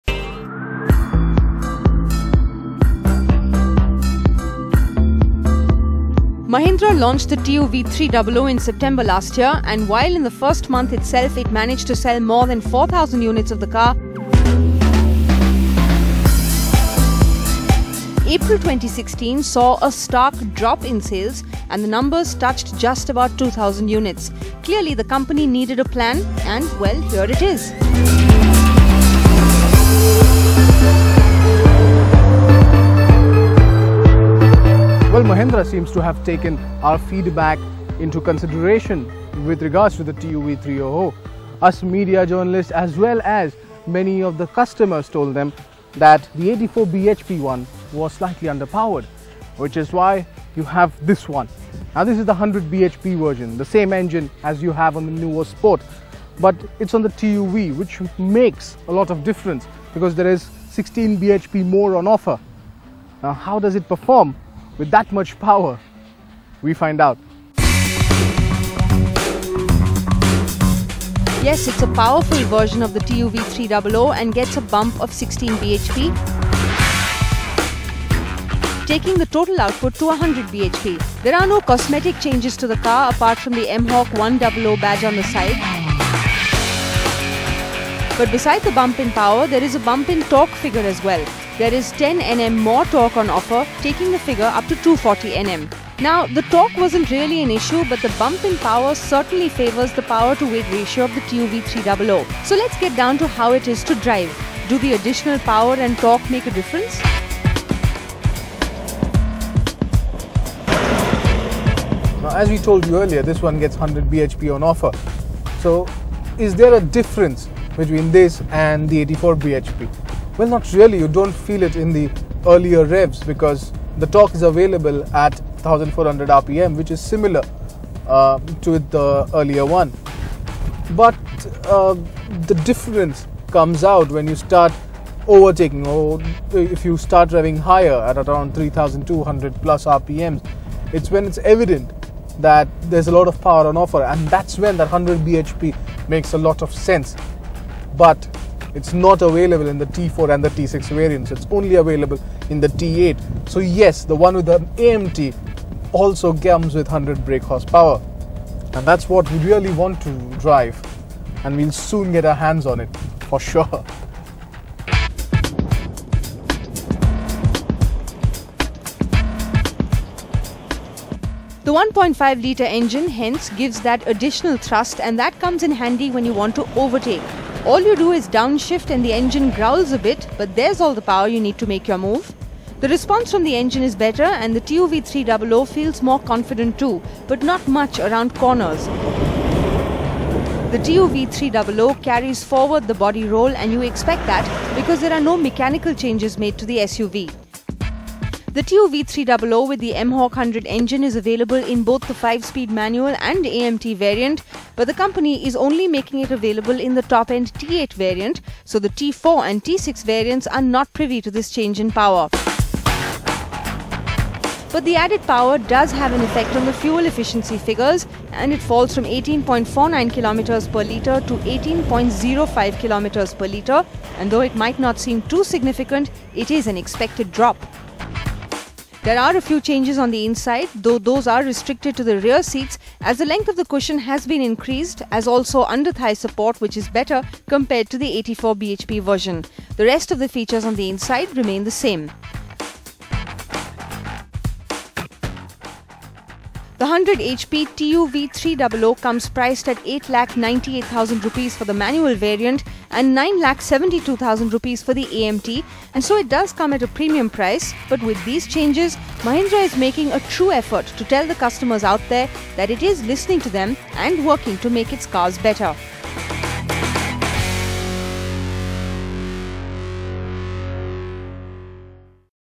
All new Mahindra TUV300 mHawk100
Mahindra has finally taken the suggestion of its customers and have come out with an upgraded engine variant of TUV300. The new variant now comes with a 100 Bhp on offer and we find out if it delivers or not?